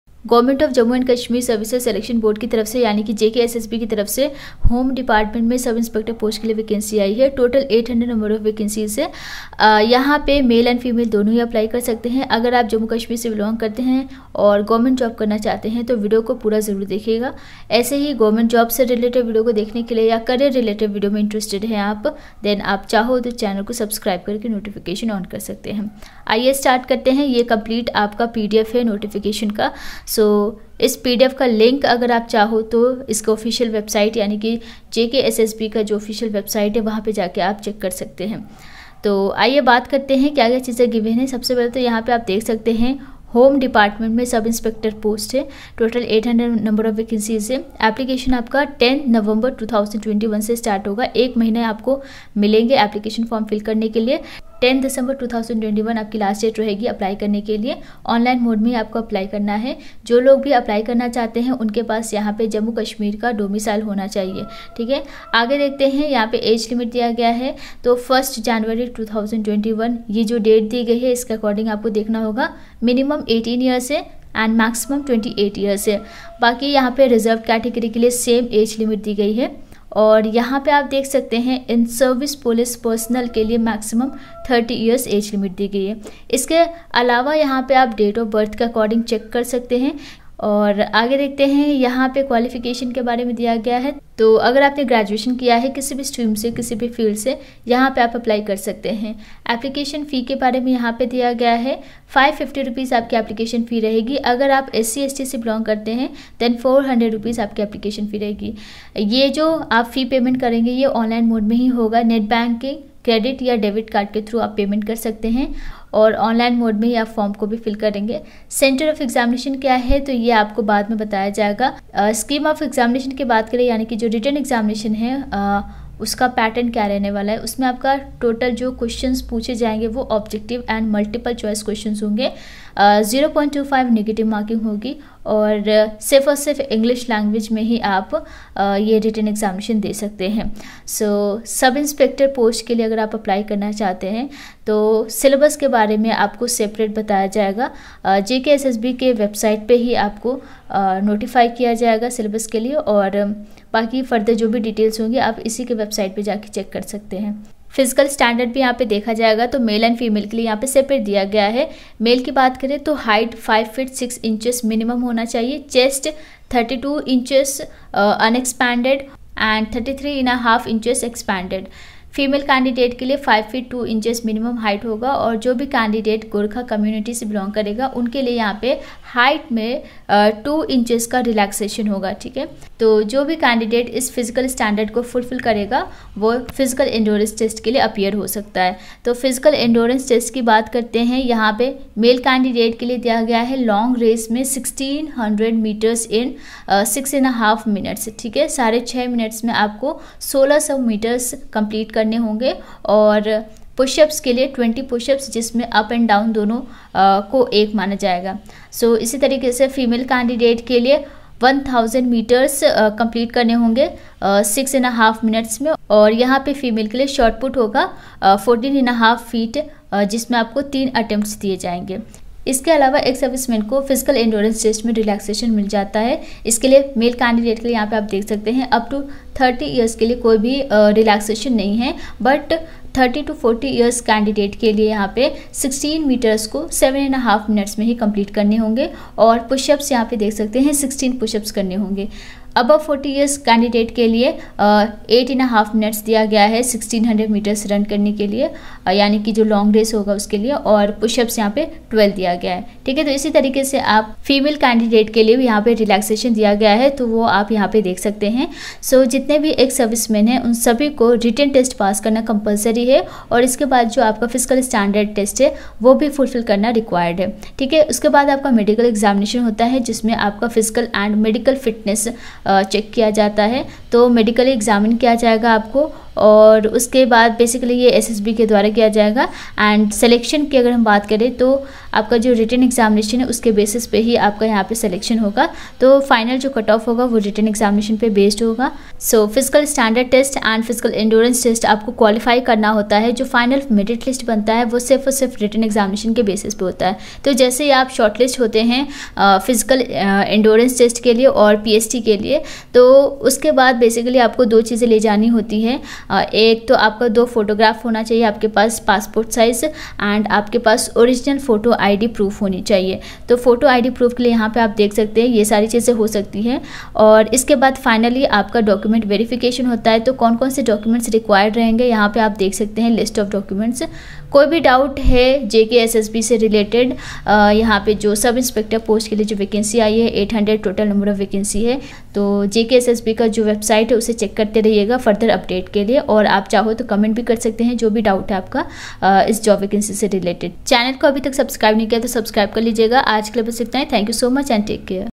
JKSSB New Recruitment 2021, JKSSB Sub-Inspector Recruitment 2021, Written Test, PET, PST
JKSSB New Recruitment 2021,
JKSSB Sub-Inspector Recruitment 2021,

0:00 Introduction
0:42 Application Date
1:06 Age Limit
1:32 Qualification
2:37 Physical standard test
3:09 Physical endurance test
4:36 Syllabus
5:17 Documents